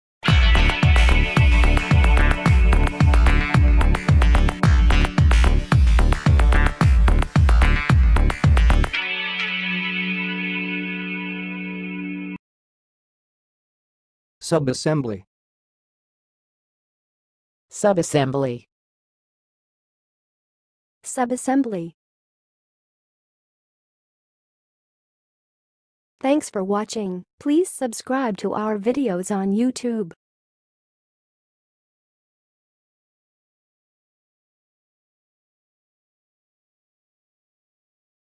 How to say "subassembly"! (High Quality Voices)
Watch in this video how to say and pronounce "subassembly"!